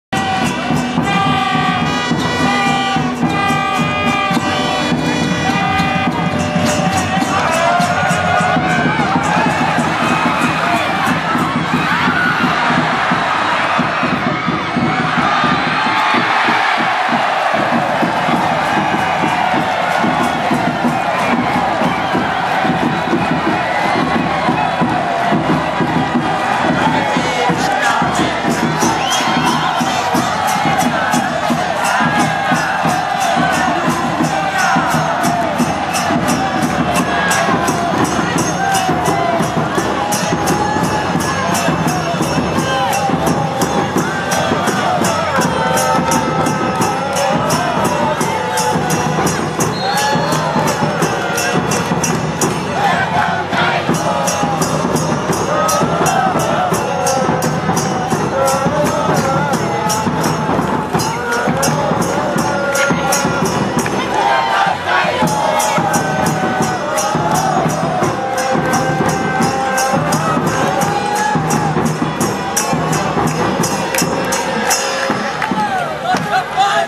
กฤษฎาเปิดให้วรัญญูแปโล่งตรังนำ 3-0
กฤษฎากระชากครึ่งสนามเปิดให้วรัญญูแปโล่งเข้าประตูตรังนำ 3-0